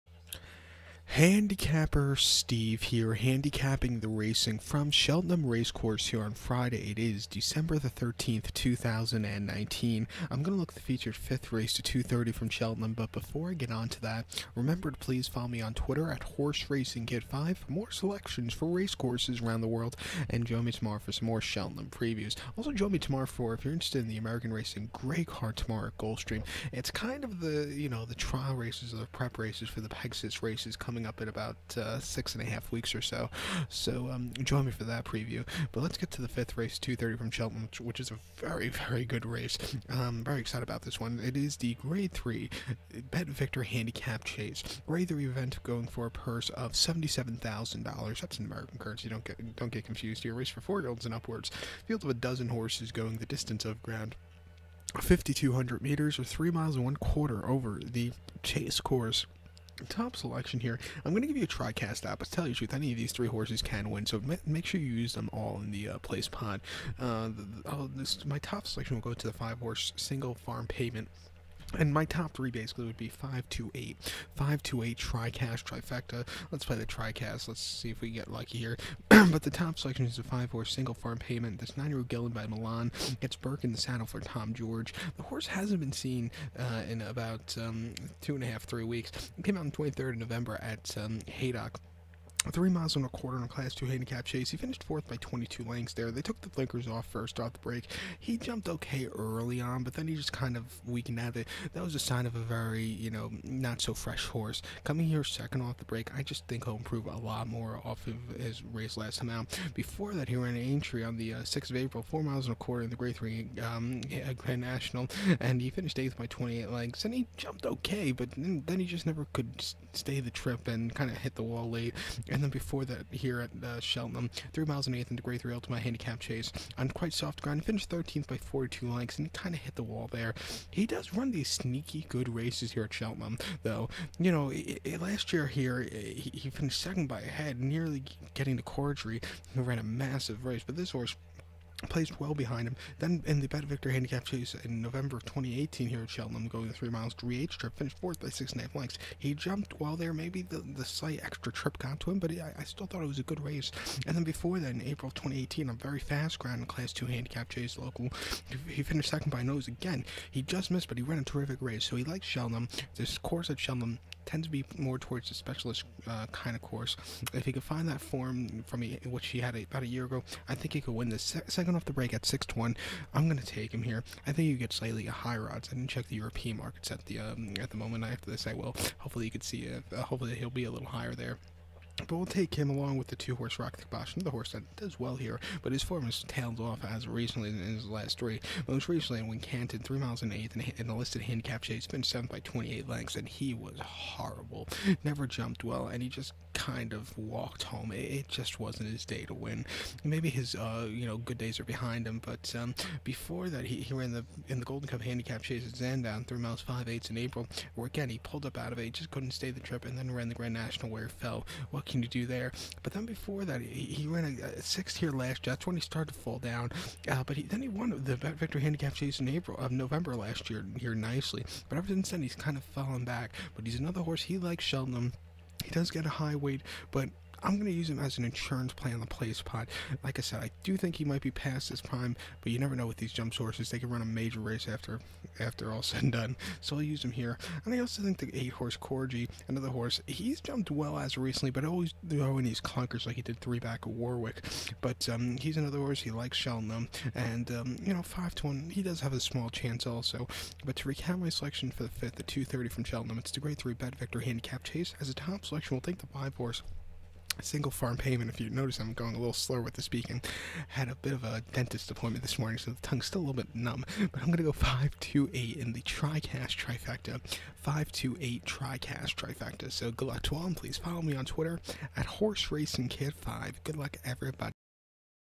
Cheltenham Racecourse Race 5 Selections Preview The Betvictor Handicap Chase 2019 12/13/2019
Selections Preview for The Betvictor Handicap Chase 2019